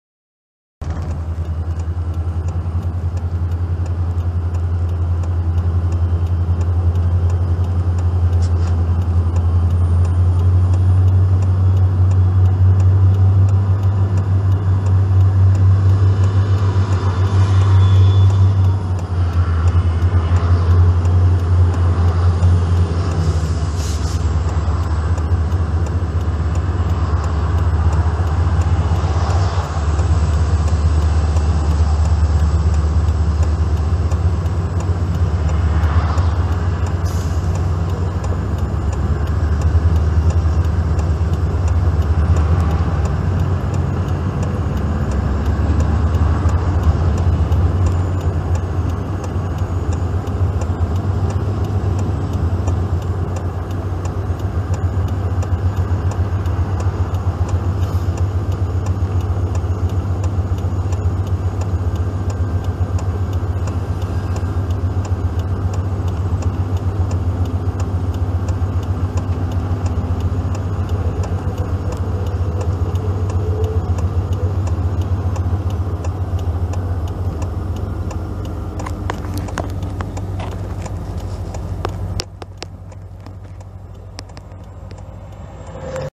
Pacing the CN at Falkland
10 MPH, more or less, along Hwy 97 just south of Falkland